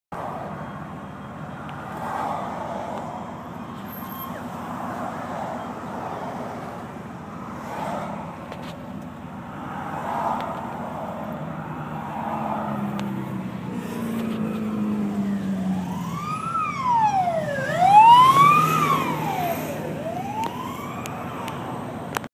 Clearwater fire rescue responding
Clearwater fire rescue CL300 and Truck 48 responding to a extercation  upgrade